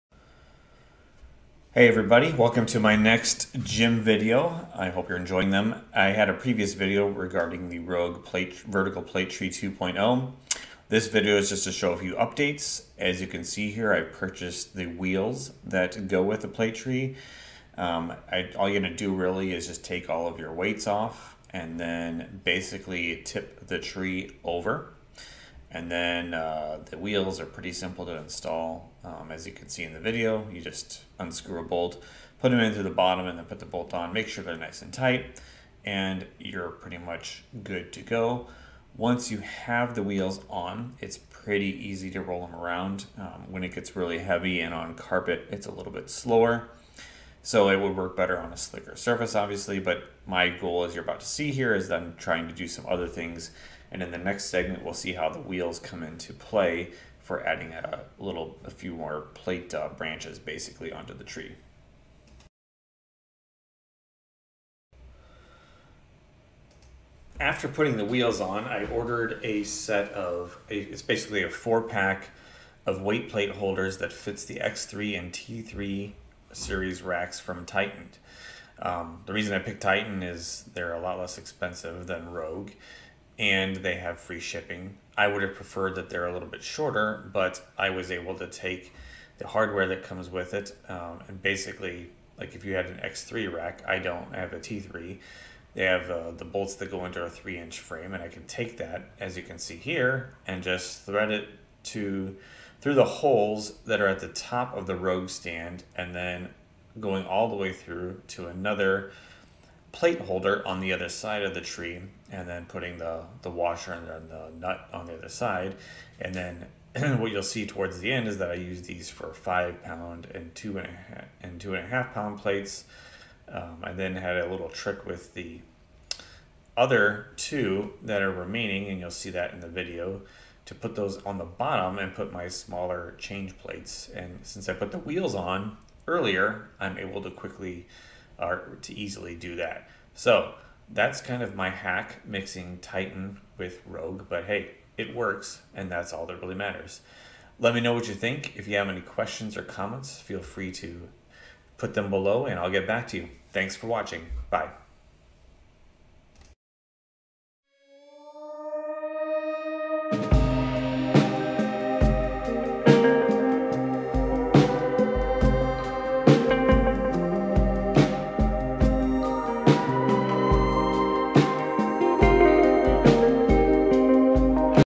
Rogue Vertical Plate Tree 2.0 Upgrades - Wheels and Titan Plate Holders!!!
In this video, I show some "hacks" I made to my Rogue Vertical Plate Tree 2.0 by adding the Rogue wheels, which gave the tree enough ground clearance to use a couple of plate holders for change plate storage.

More importantly, the other two plate holders can be attached to the upper holes on the Rogue Plate Tree 2.0 to attach Titan plate holders.  Who knew?!?

Here is a link to the Titan plate holders, which worked perfectly with the Rogue Plate Tree.